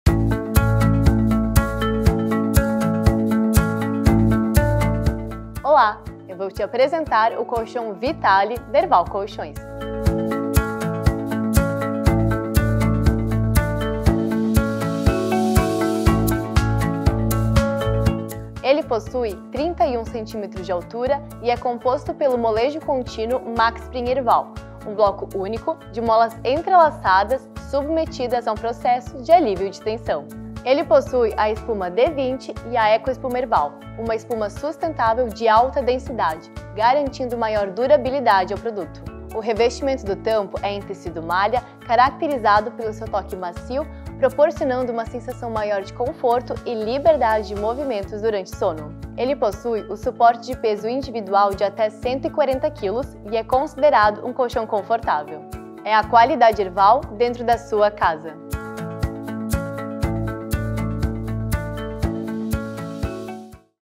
Colchão Vitalle - Herval Colchões
O colchão Vitalle Maxspring foi desenvolvido para quem busca um conforto uniforme e personalizado em seu colchão Herval. Com o exclusivo sistema de molas Maxspring, ele se adapta perfeitamente às necessidades do seu corpo durante o sono, proporcionando suporte ideal. Além disso, seu revestimento em tecido de malha oferece não apenas um toque macio e aconchegante, mas também um visual sofisticado, com a elegância característica da Herval.